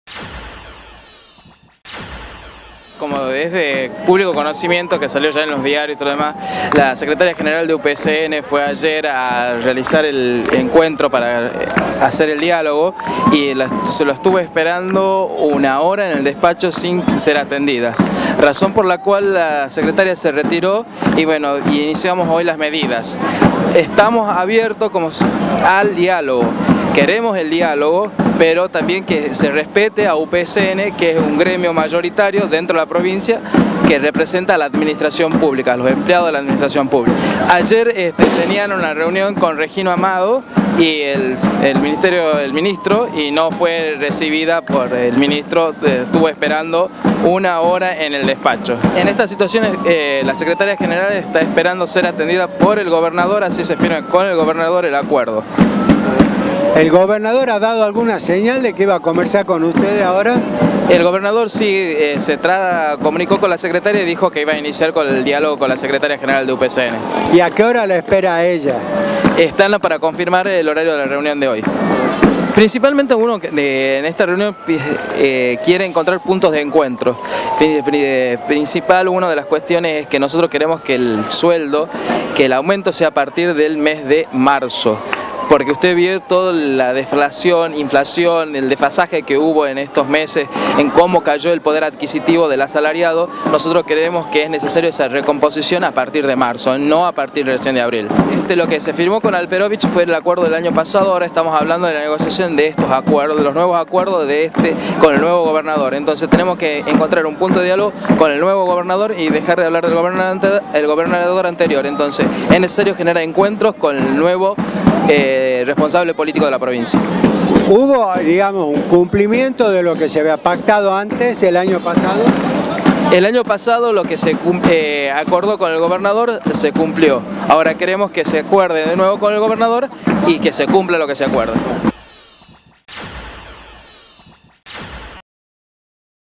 Por un reclamo salarial UPCN generó un caos en el centro tucumano
La Unión del Personal Civil de la Nación (UPCN) dispuso este miércoles una serie de cortes de calle en diferentes puntos del microcentro, lo que provocó un verdadero dolor de cabeza para conductores y transeúntes.